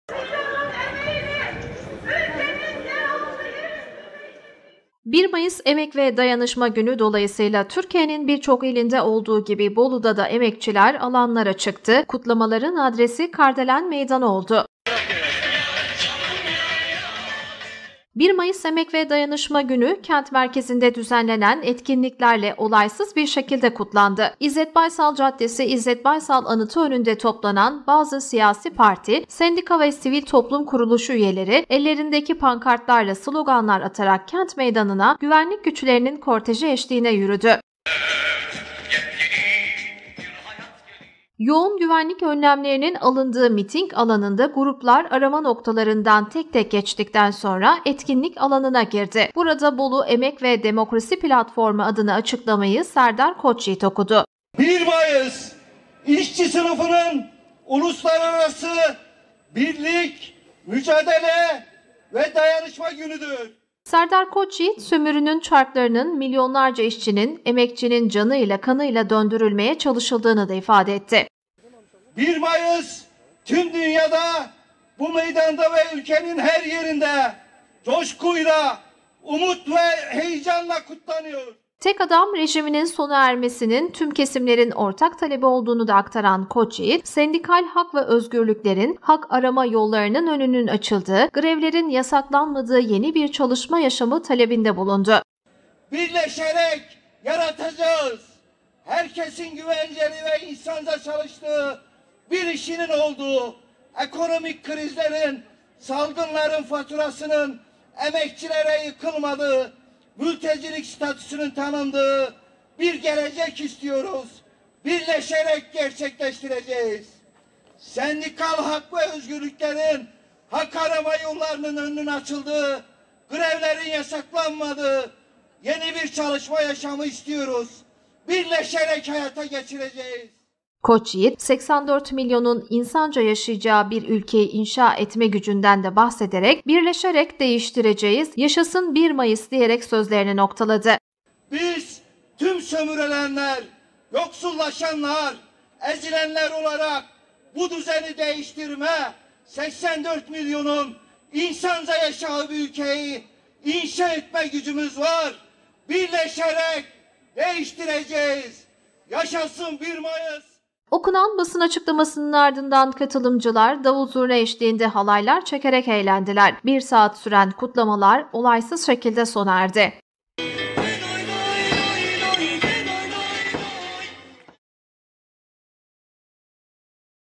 1 MAYIS COŞKUSU BOLU’DA DA YAŞANDI (01.05.2022)
1 Mayıs Emek ve Dayanışma Günü dolayısıyla Türkiye'nin birçok ilinde olduğu gibi Bolu’da da emekçiler alanlara çıktı. Bolu’da kutlamaların adresi kardelen meydanı oldu.